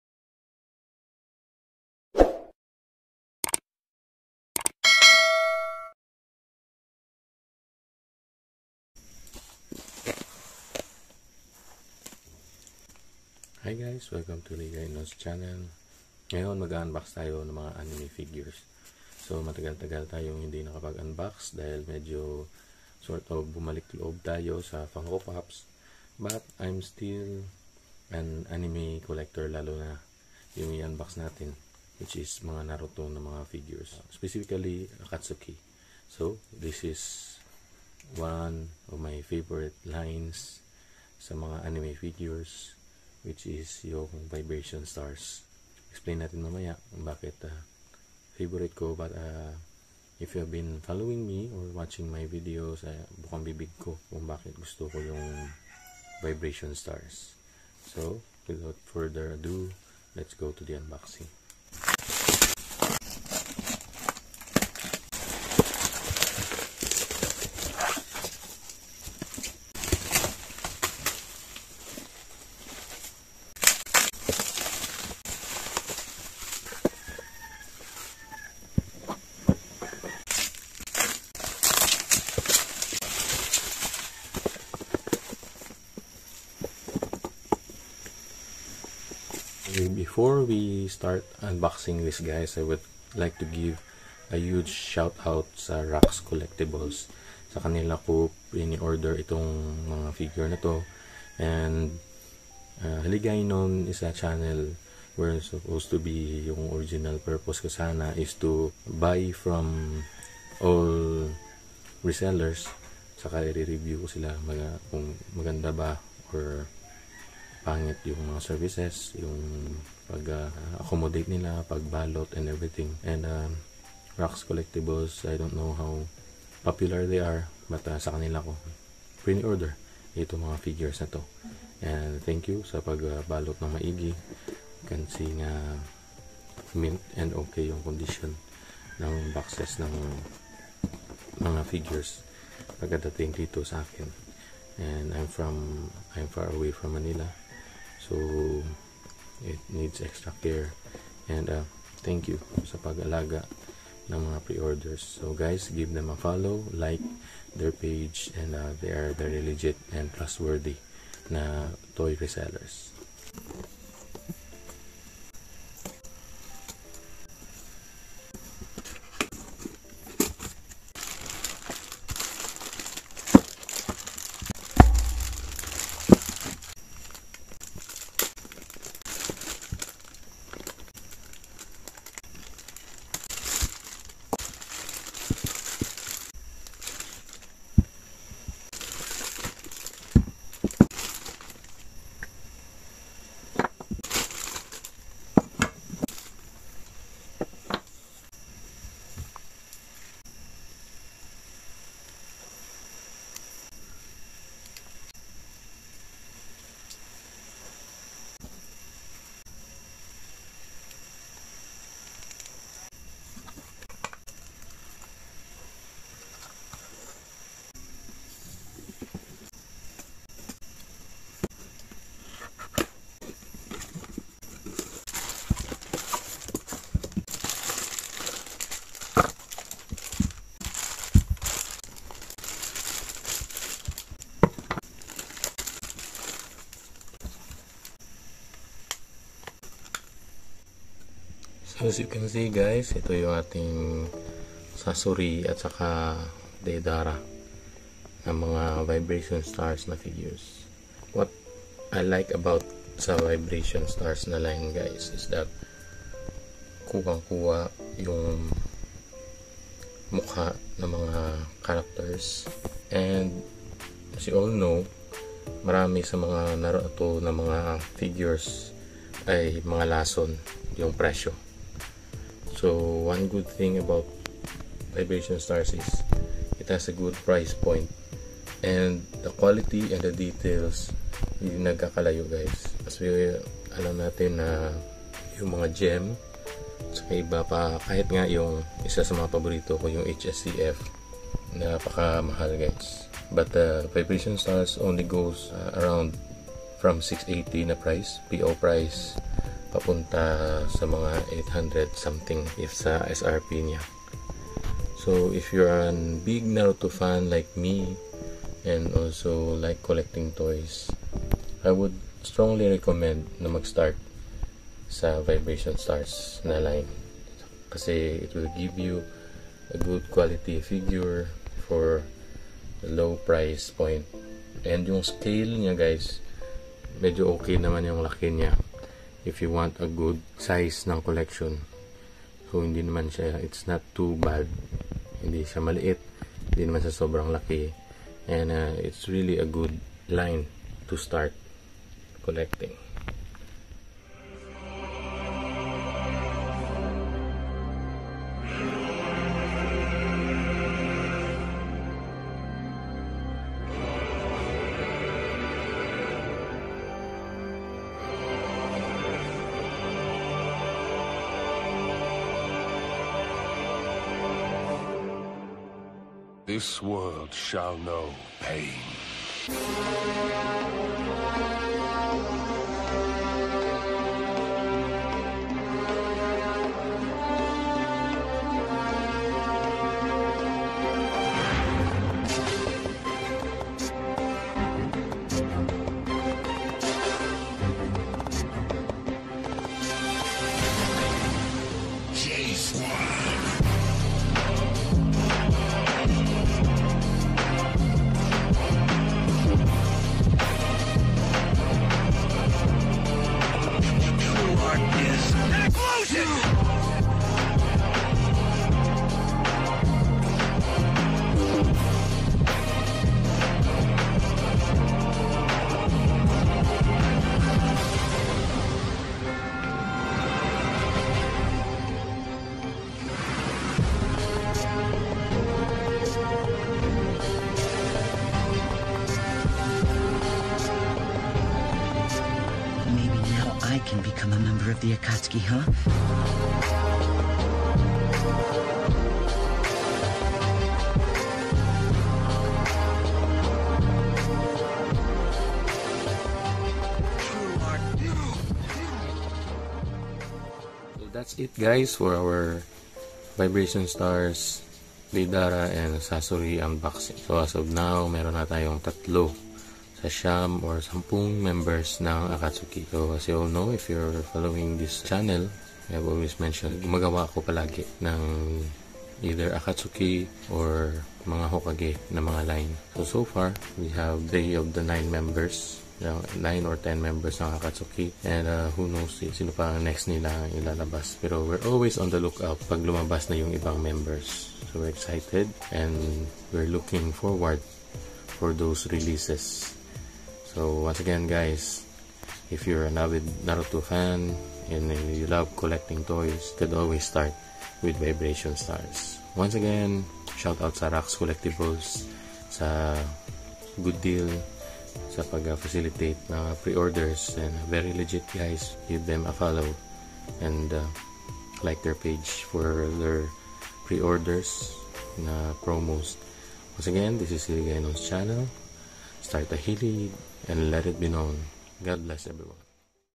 Banpresto - Bandai Namco | Vibration Stars Sasori and Deidara Unboxing
Unboxing of probably one of the most underrated anime figure line. Vibration Stars has good details, good size scale, and best of all, doesn't break the bank.

**Not a paid endorsement